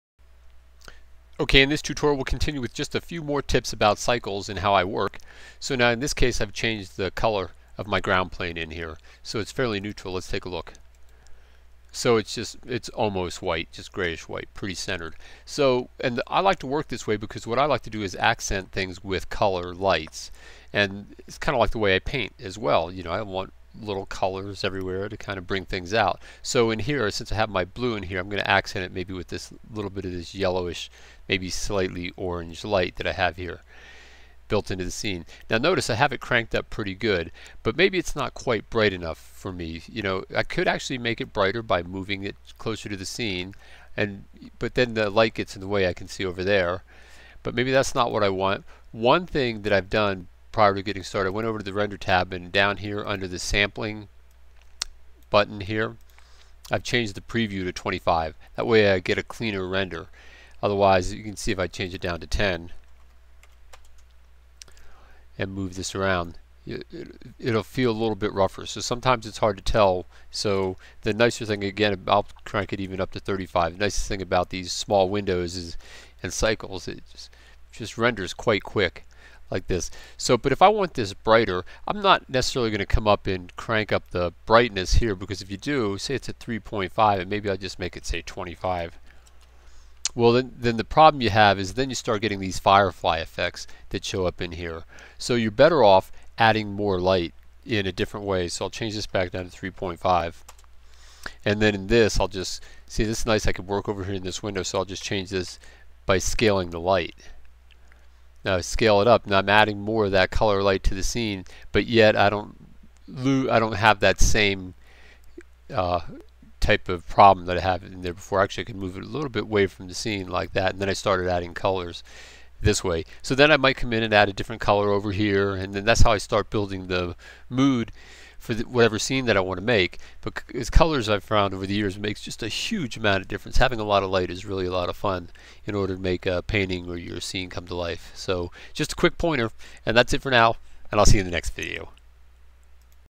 Blender Tutorial - Avoiding Graininess in your Cycles Rendering
In this Blender tutorial, I show you one method that I use to avoid graininess in my Cycles renders.